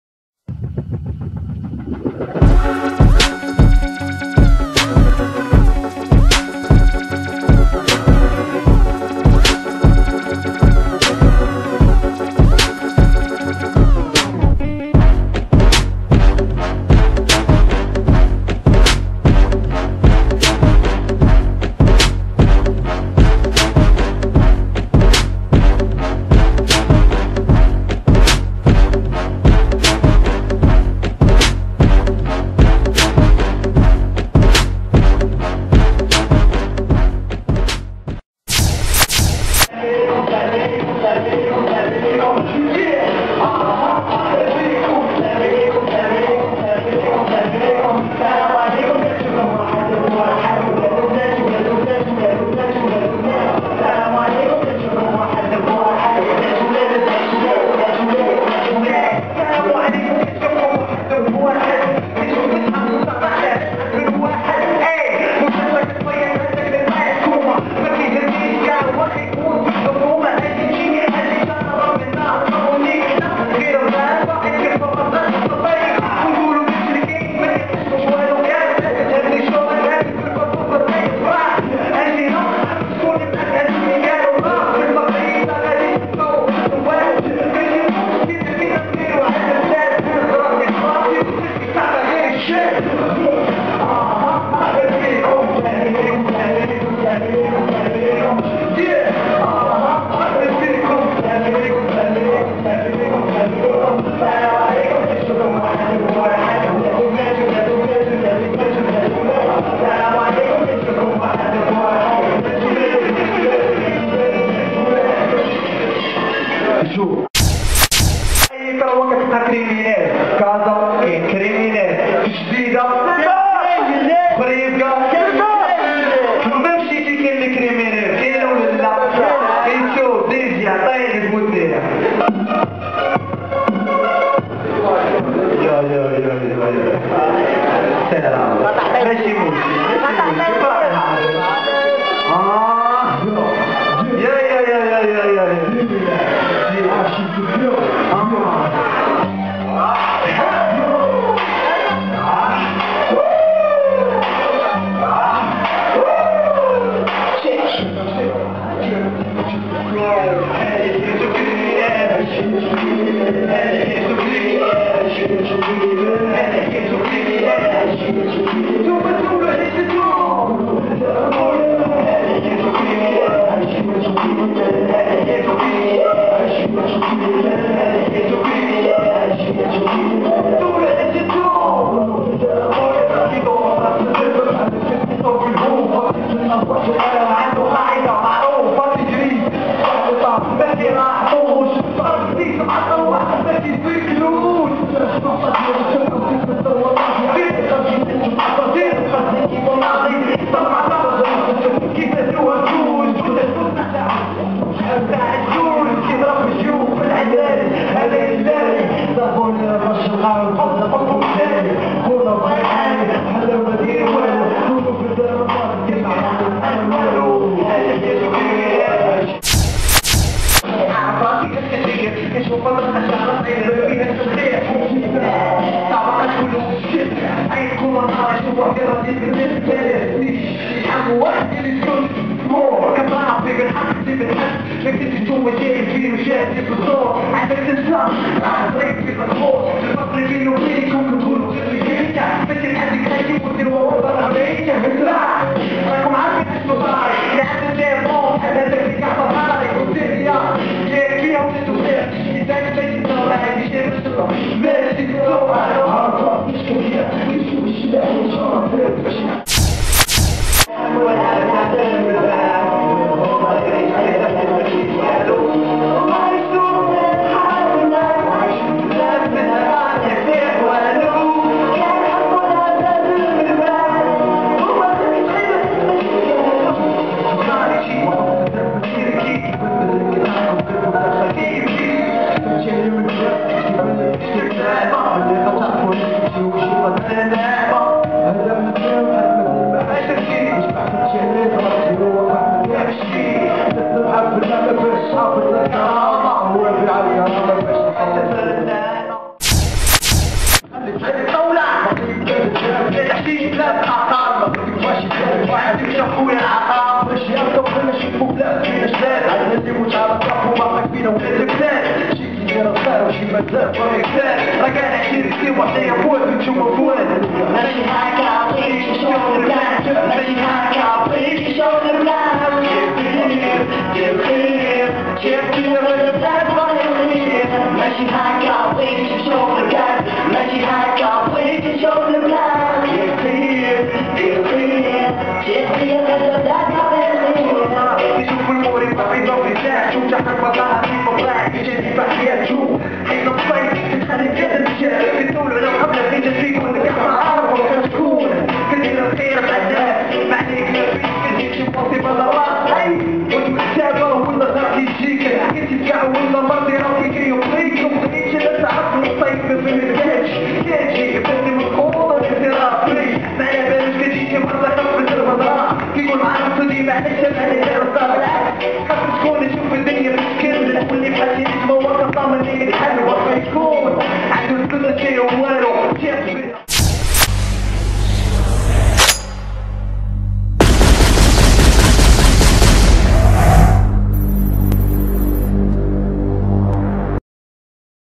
HS CREW Live AU - B-Rock de Casablanca 2013
HS CREW Sur scéne Live au B-Rock de Casablanca .
chi haja Testahel Tchofouha Peace